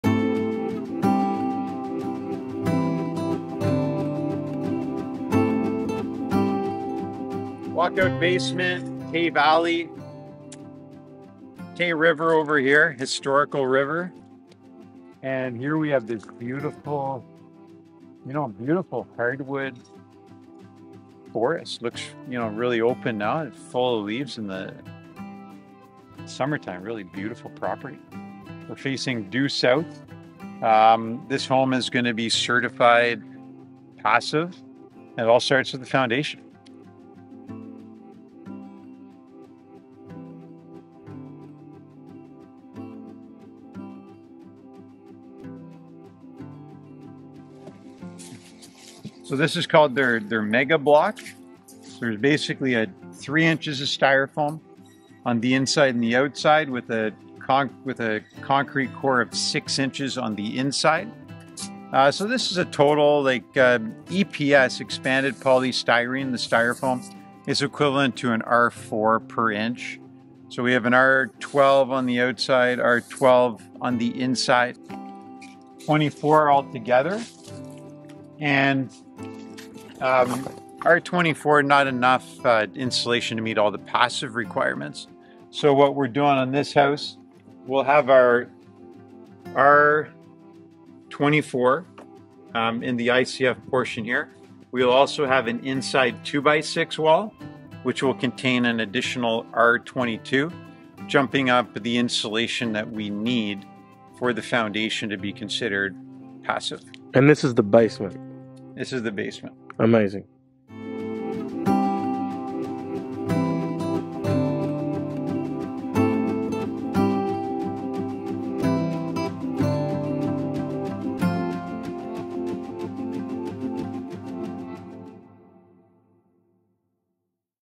Passive House - Basement Insulation with Paul Kealey
Paul take us to Perth, Ontario where Ekobuilt is in the process of installing a Walk-out basement!
Paul explains in detail how a passive-basement comes together using modern technologies to ensure even the basement is at passive-certified standard.

We're now on Tik Tok: @ekobuiltpassivehouse